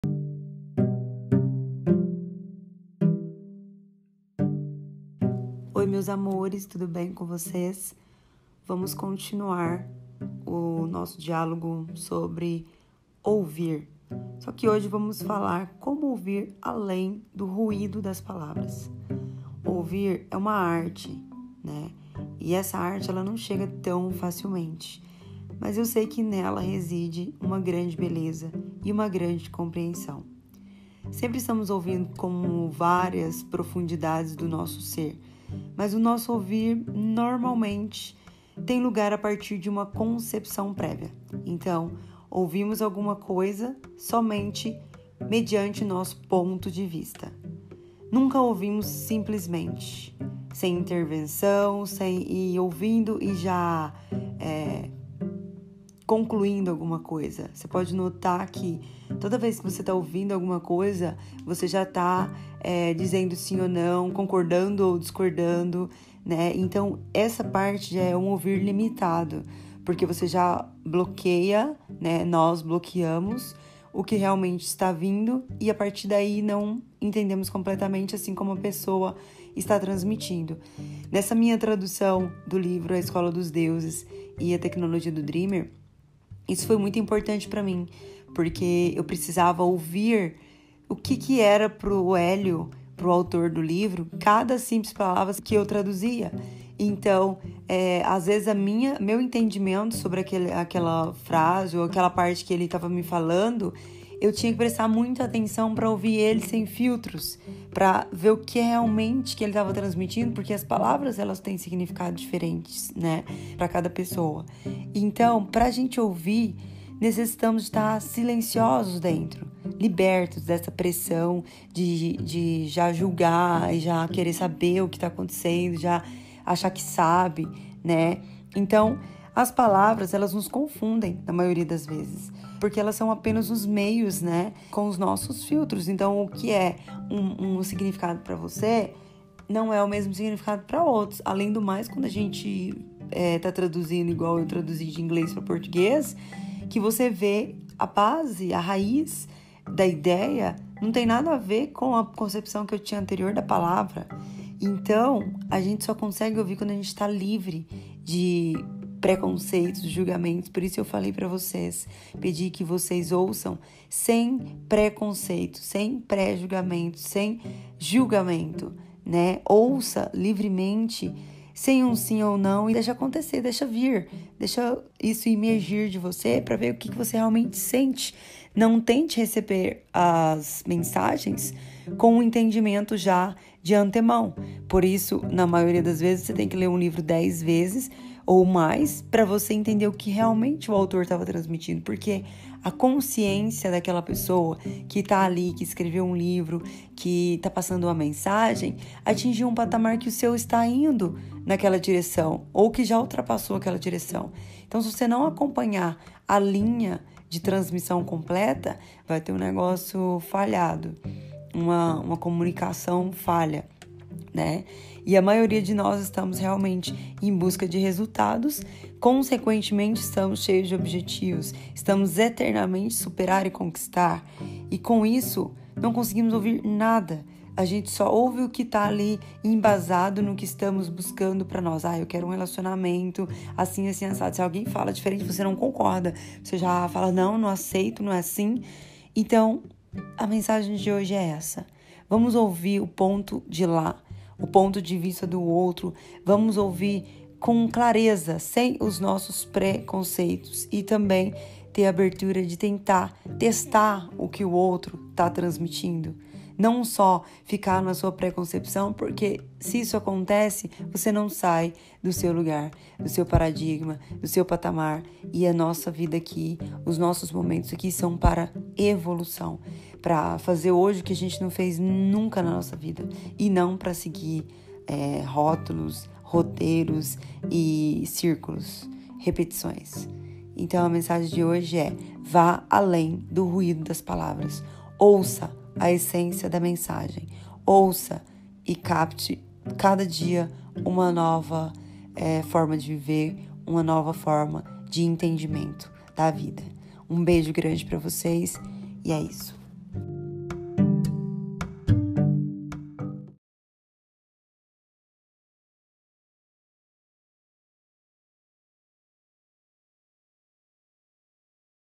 Dia 3 | Como ir ALÉM do ruído das PALAVRAS ? Quantos livros já leu e conseguiu resultados indicado?
Como ultrapassar as barreiras limitantes das palavras preconcebidas?

Para ir além do ruído das palavras, é necessário ouvir e isso é uma grande arte e que não chega facilmente, reside no ouvir uma grande beleza e uma grande compreensão. Ouvimos com várias camadas do nosso SER, mas o nosso ouvir se inicia com base de uma de nossas preconcepções ou de um determinado ponto limitante de vista.

Nunca ouvimos simplesmente: estamos sempre com um filtro, um bloqueios, pensamentos, conclusões e preconceitos... Para ouvir necessitamos de uma quietude interior, de estarmos libertos da pressão da adquirir alguma coisa, de uma atenção dispersa. Este estado de atenção passiva é capaz de ouvir o que está além da nossa conclusão verbal.

As palavras nos confudem, elas são apenas os meios externos de comunicação; mas para se conversar com inteira profundidade, para ir além do ruído das palavras, deve haver no ouvir uma passividade alerta.

A maioria de nós andamos atrás de resultados, da realização de objetivos, estamos eternamente a superar um obstáculo e a conquistar alguma coisa, e com tal não conseguimos ouvir nada. É somente no ouvir que escutamos a melodia das palavras.

Me acompanhe pelo seu caminho, se eu puder te aconselhar em algo basta me enviar um email
Nós somos dreamers juntos

Seja Criador e Não Criado
Vem aprender comigo!!